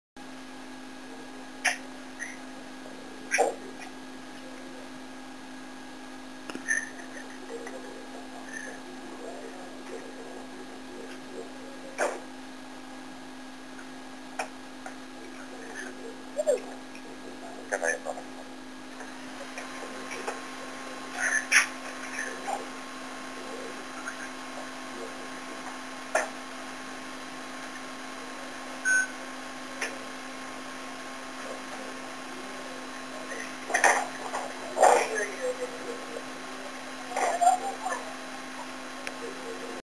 borracho vs autobus - tranvia / drunk vs bus - tram
graciosos y divertido video en el que un borracho intenta subir a un tren o autobus o tranvia y vean si lo consigue o no. es graciosisimo y super chistoso, chistosos, comedia te partiras de risa XD
drunk versus bus or tram